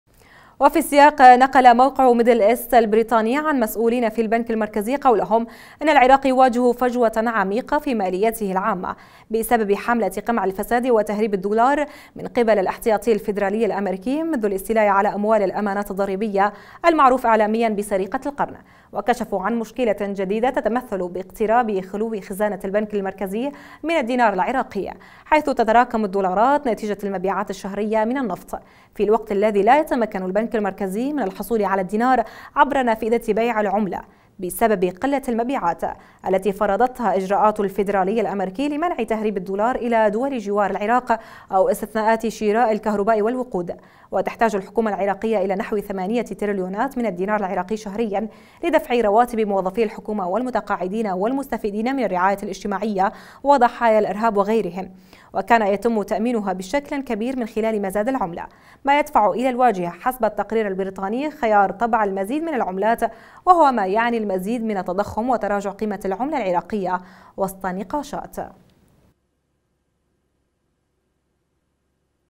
تقریر بریطاني: العراق يواجه نقصاً حاداً في الدينار ويكافح لدفع الرواتب
وفي السياق نقل موقع ميدل أيست البرطاني عن مسؤولين في البنك المركزي قولهم: إن العراق يواجه فجوة عميقة في ماليته العامة بسبب حملة قمع الفساد وتهريب الدولار من قبل الاحتياطي الفيدرالي الأمريكي منذ الاستيلاء على أموال الأمانات الضريبية المعروف إعلاميا بسرقة القرن، وكشفوا عن مشكلة جديدة تتمثل باقتراب خلو خزانة البنك المركزي من الدينار العراقي، حيث تتراكم الدولارات نتيجة المبيعات الشهرية من النفط، في الوقت الذي لا يتمكن البنك المركزي من الحصول على الدينار عبر نافذة بيع العملة بسبب قلة المبيعات، التي فرضتها اجراءات الفيدرالي الأمريكي لمنع تهريب الدولار الى دول جوار العراق أو استثناءات شراء الكهرباء والوقود، وتحتاج الحكومة العراقية الى نحو ثمانية ترليونات من الدينار العراقي شهريا لدفع رواتب موظفي الحكومة والمتقاعدين والمستفيدين من الرعاية الاجتماعية وضحايا الإرهاب وغيرهم، وكان يتم تأمينها بشكل كبير من خلال مزاد العملة، ما يدفع الى الواجهة حسب التقرير البريطاني خيار طبع المزيد من العملات وهو ما يعني المزيد من التضخم وتراجع قيمة العملة العراقية وسط نقاشات.